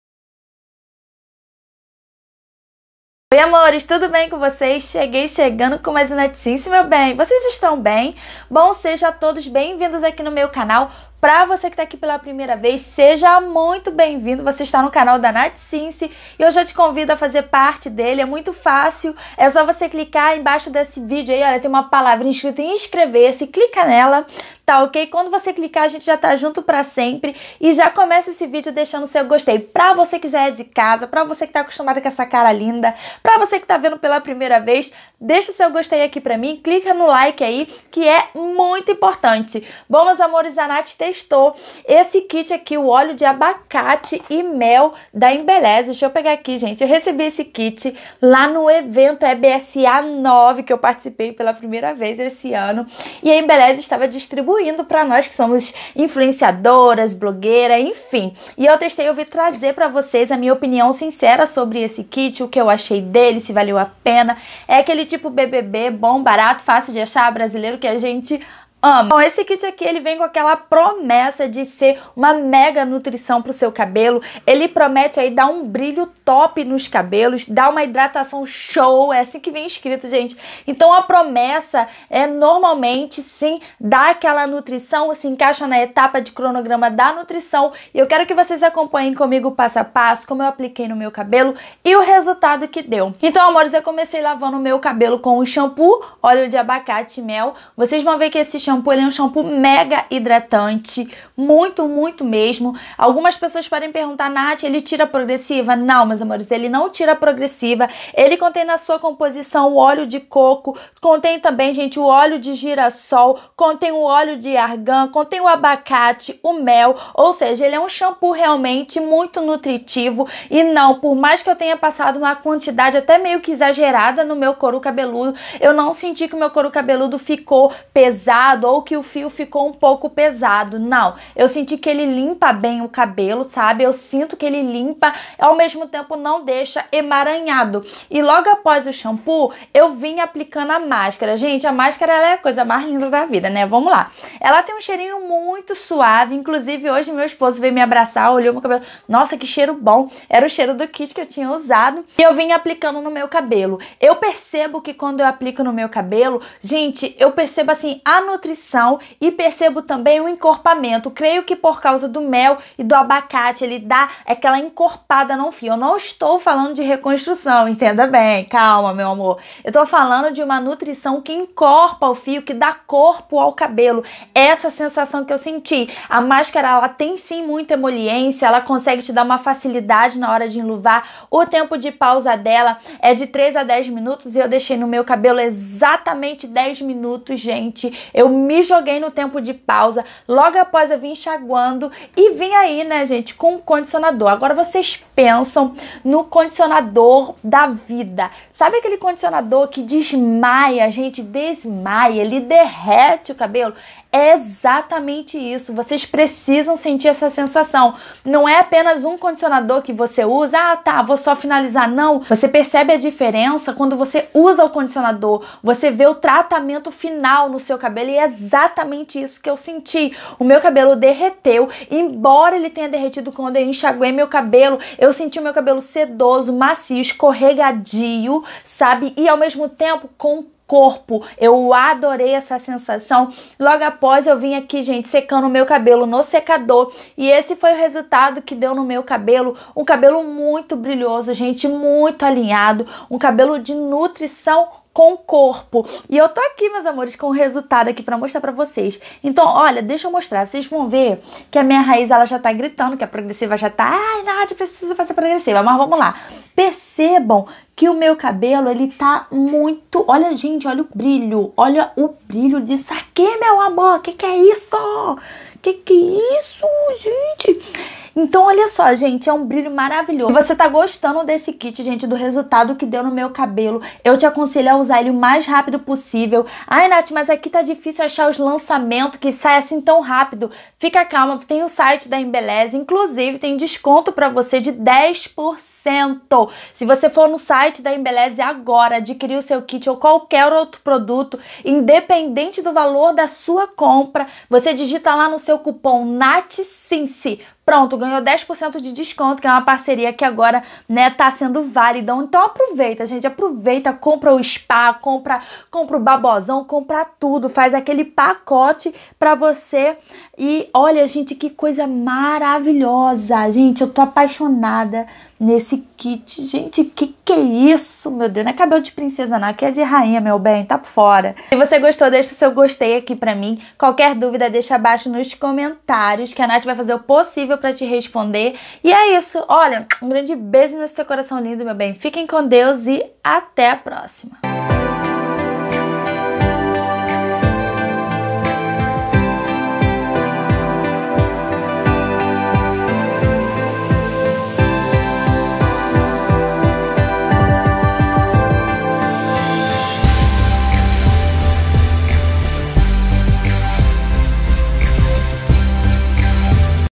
TESTEI ABACATE E MEL LANÇAMENTO EMBELLEZE💛
Amores usei o kit completo de Abacate e Mel da Embelleze, essa linha promete hidratação, brilho e nutrição profunda.

  💛 É SÓ DIGITAR O CUPOM: natysince
Lembrando que o desconto é independente do valor de sua compra.

💛 Contém em sua composição, uma gama de óleos, como o de coco, linhaça, azeite, macadâmia, e claro o agente principal abacate e mel.
💛  O mel restaura e fortalece os fios e o abacate e rico em ácidos graxos.
💛 Não é liberado pois contém parafina liquida